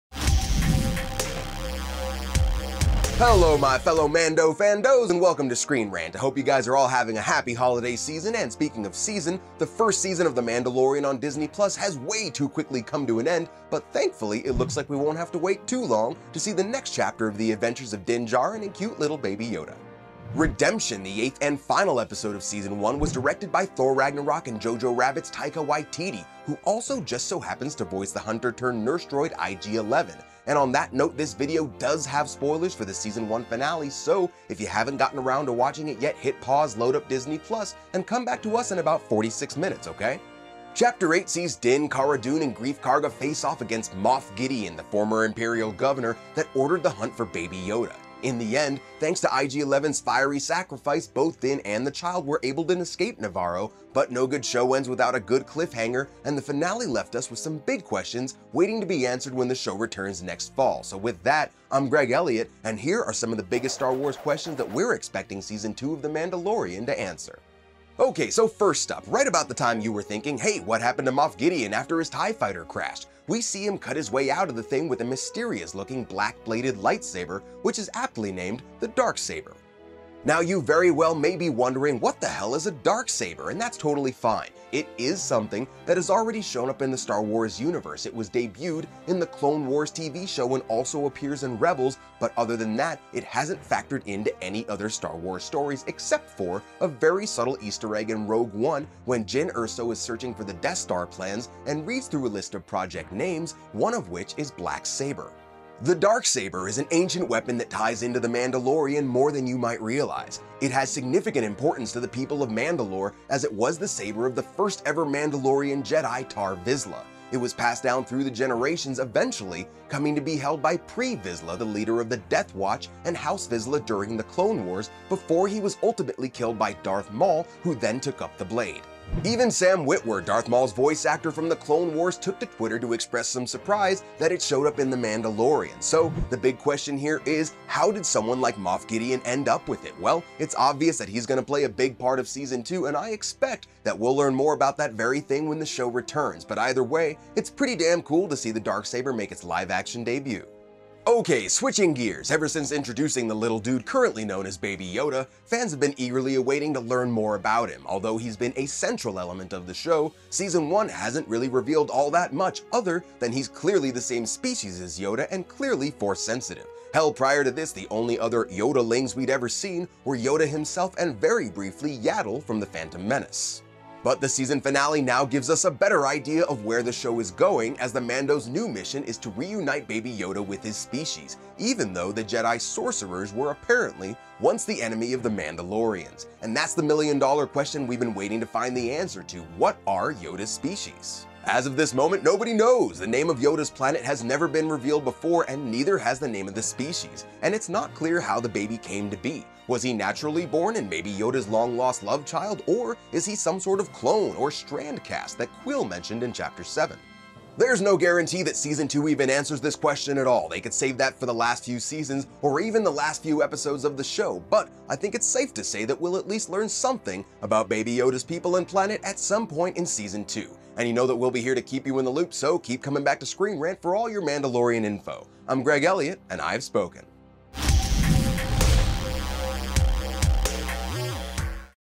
The Mandalorian Season 2 Will Answer BIG Star Wars Questions
The Mandalorian season 1 finale "Chapter 8: Redemption" sets up the show's second season to address some of Star Wars' biggest mysteries.